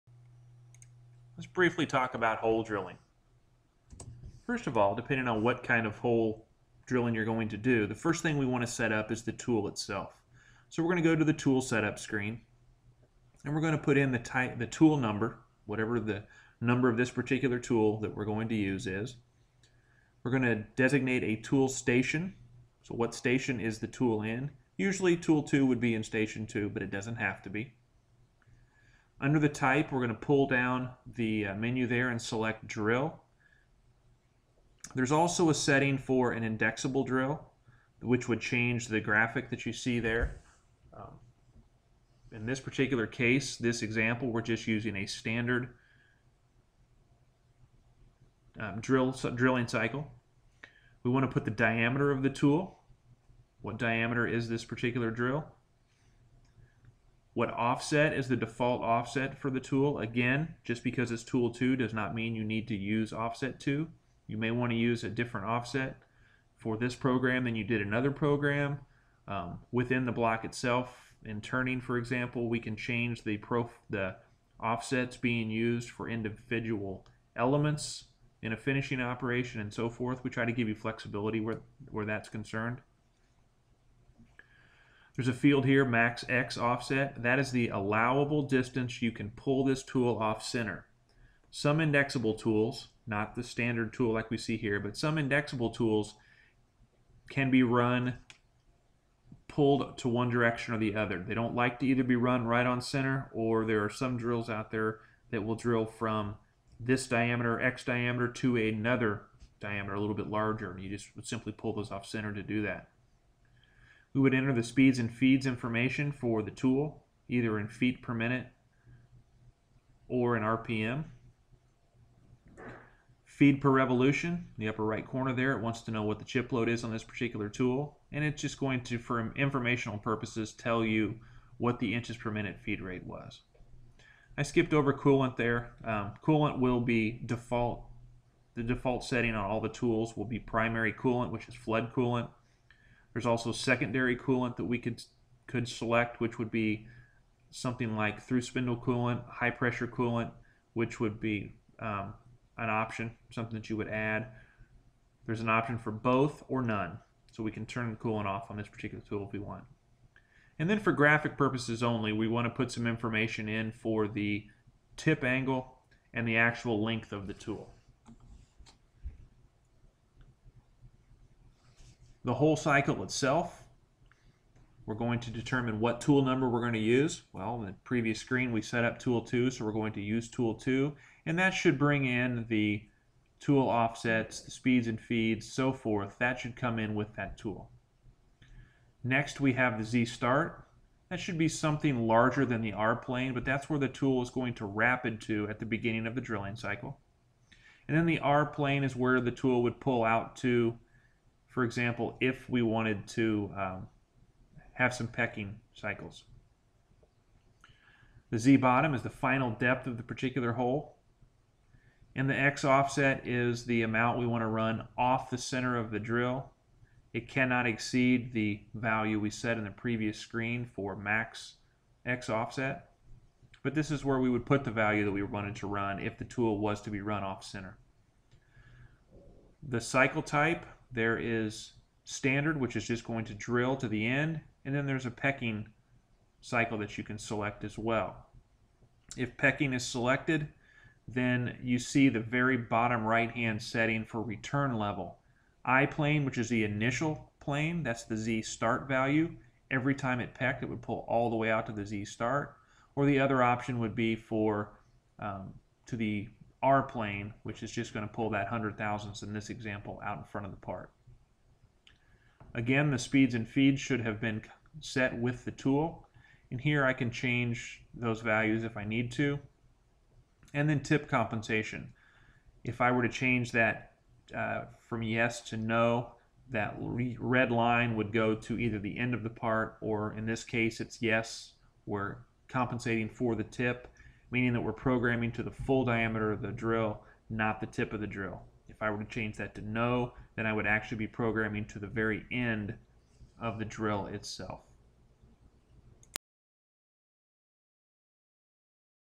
Hole Drilling Cycle - Hurco Lathe Training - Max4 Version 9
Programming a hole drilling cycle using the Holes data block.